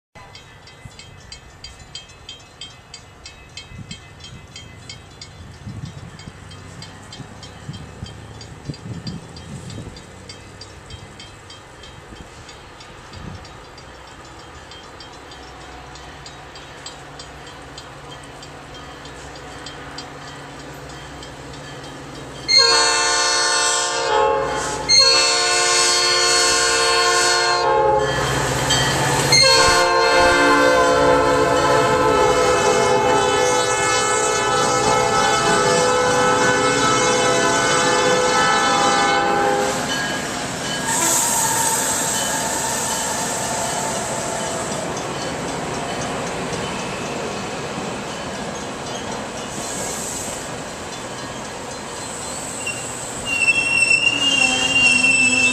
Metra F40PH-3M 103 MDNL 2609 Lake Forest
Metra F40PH-3M 103 brings MDNL 2609 into its station Stop at Lake Forest Recorded on October 9, 2010 using a Flip Video camcorder.